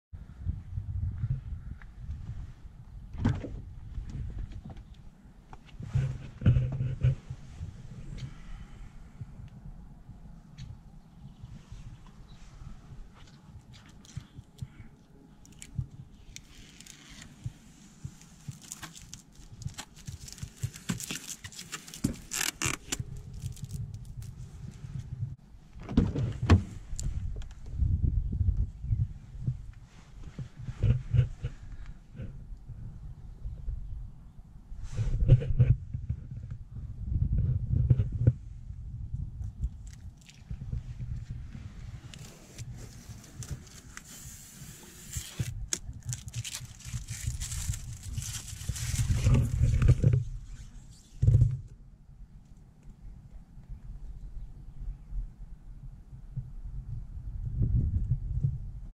750 HP AMG C63 Coupé - Removing Door Sill Protectors
After 2 years it was time ro remove the Protectors

C63 Coupé:
750 HP / 870 NM
- Turbos by Dynotech
- Downpipes by Dynotech